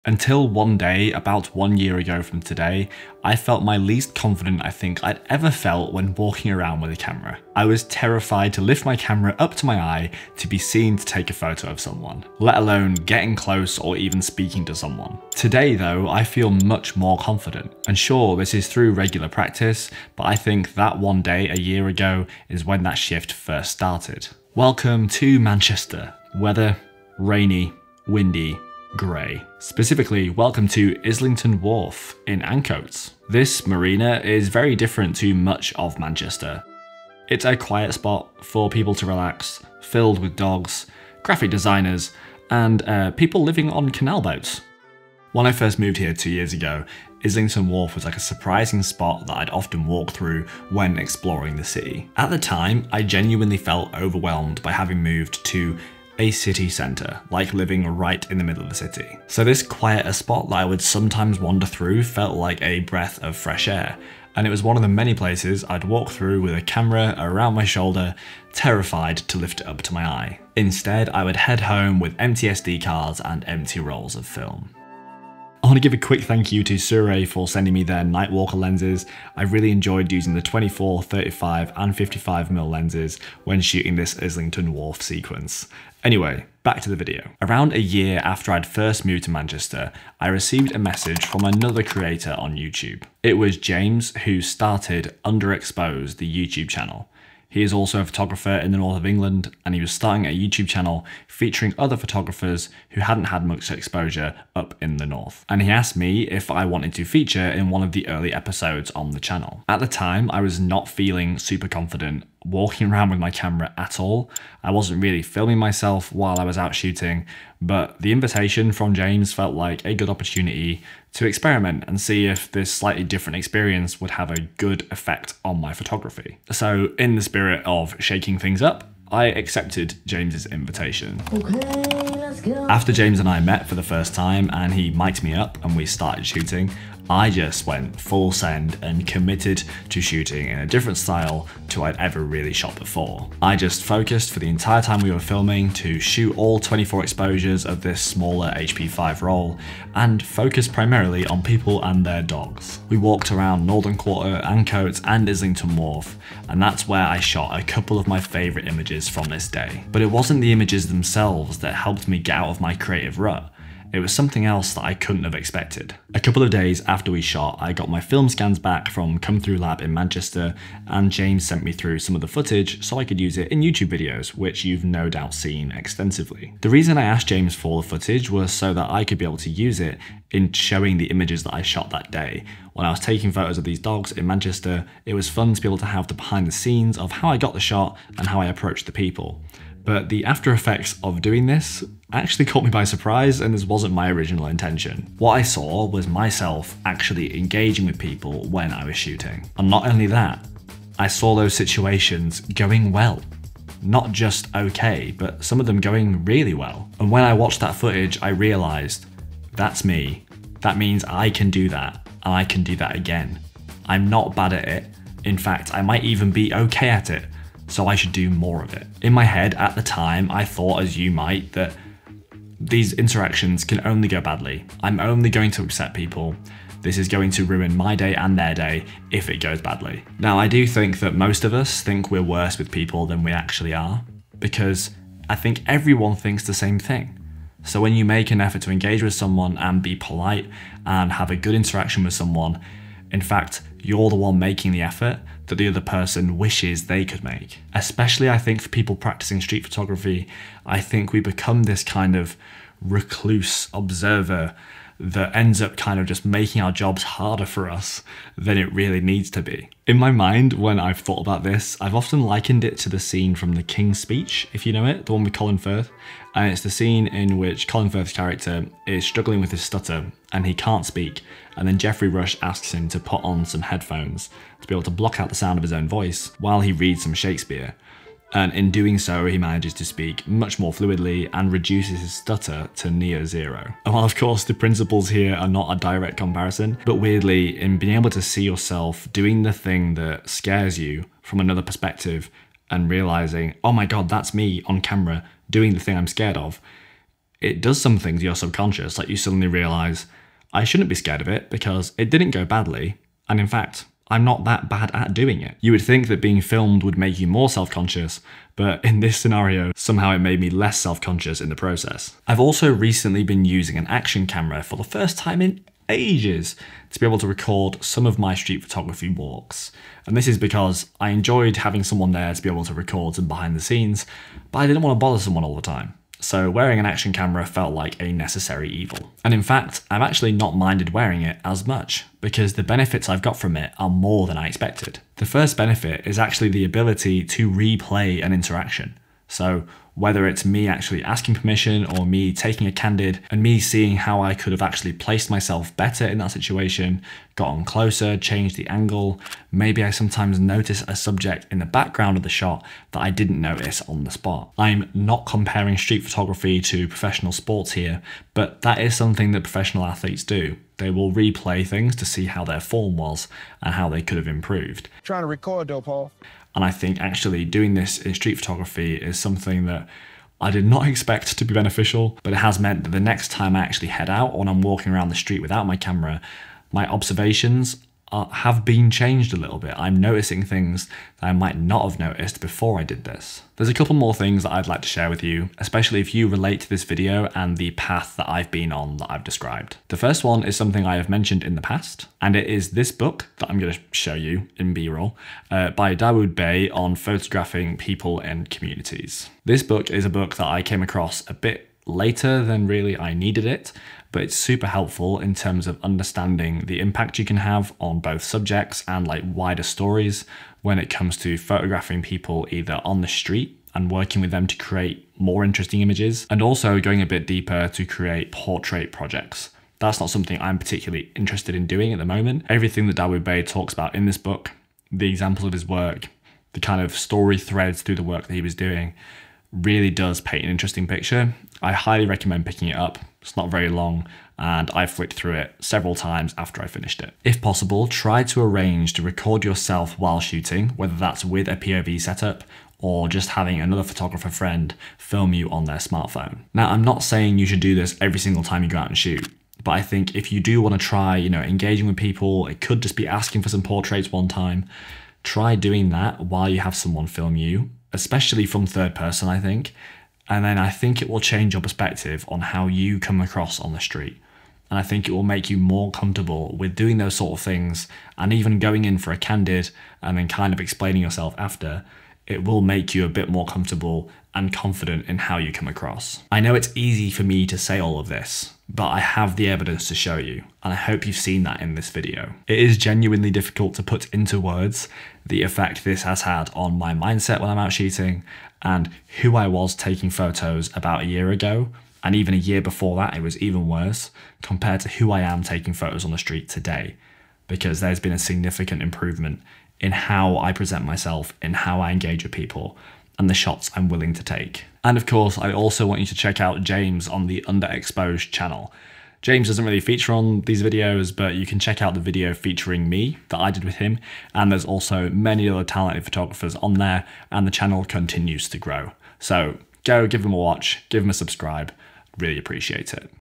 Say Goodbye to Street Photography Anxiety
Street photography may be the most challenging form of photography, not just on a technical level, but on a social and personal level. Many street photographers enjoy the art form, they enjoy the tools, but may not be super-extroverted. That is my problem, I'm okay with people but my anxiety gets in the way of taking the shots I'd love to take. In the past 2 years, I am happy with the progress I've made, going from an anxious observer to an active participant on the street.

Check out MPB here and grab a used camera deal!

0:00 - My lack of confidence
0:24 - Where it happened
1:27 - Photography community
2:11 - My first time doing this
3:15 - What this taught me
4:20 - Doing what scares you
5:12 - Studying your process
6:24 - You should read this
7:17 - Record your photography
7:56 - My EVIDENCE
8:25 - Go watch undrxpsd